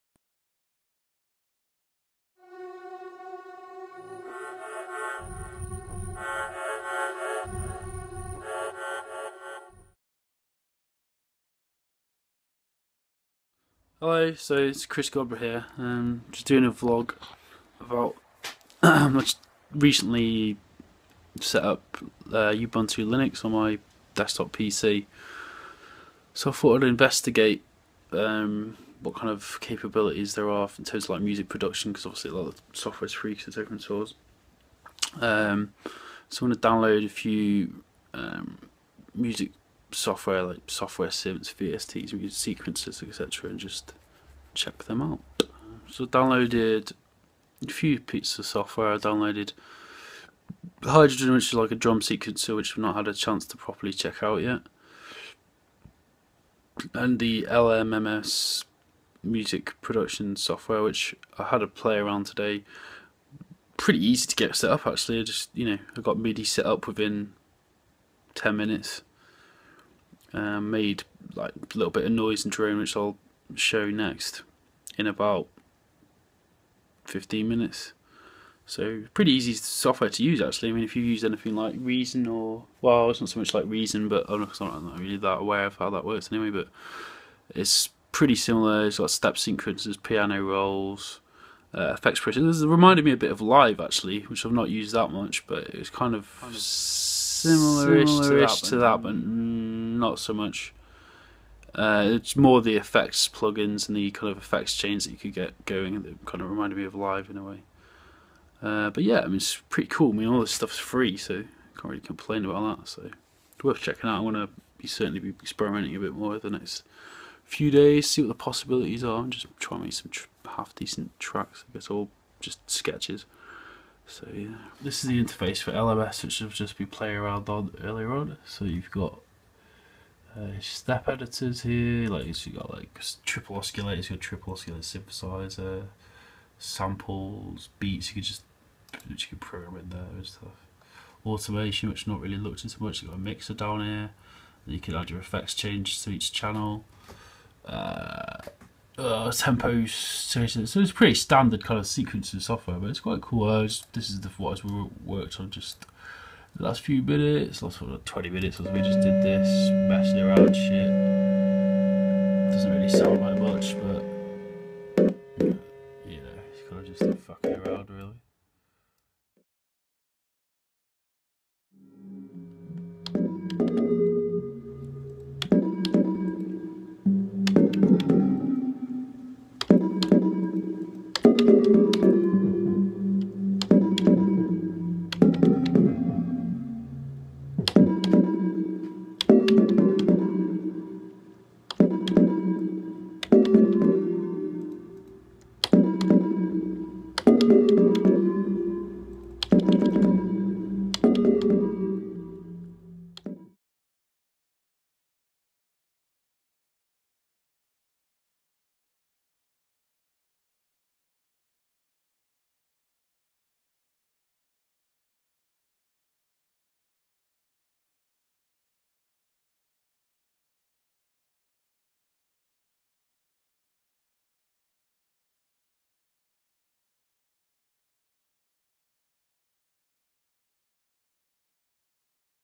Vlog - Trying out LMMS Music Sequencer on Linux
First impressions using  LMMS music sequencer on linux - with Midi Korg Nanokey